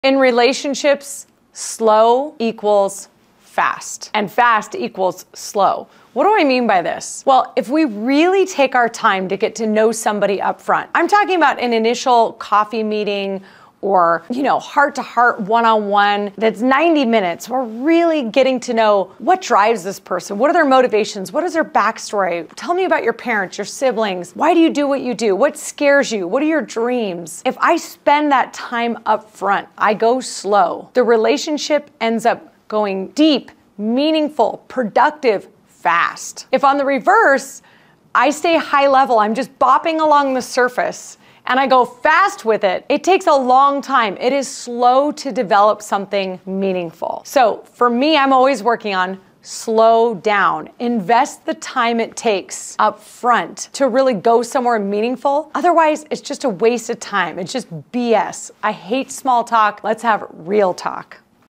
Slow Equals Fast
In relationships, the concept of 'slow equals fast, and fast equals slow' holds true. Let me explain. When we take our time upfront, investing in deep conversations, truly getting to know someone's passions, motivations, and their unique story, magic happens. 💖☕️ Spending 90 minutes over coffee, diving into their upbringing, fears, dreams, and desires, lays the foundation for a deep, meaningful, and fast-moving connection.

On the other hand, if we stay surface-level, rushing through interactions, it takes a long time to develop something truly meaningful. ⏳🌊 For me, it's all about slowing down, investing the time required to build genuine connections. Anything less feels like a waste of time and small talk.

Let's skip the BS and engage in real talk, where the magic truly happens.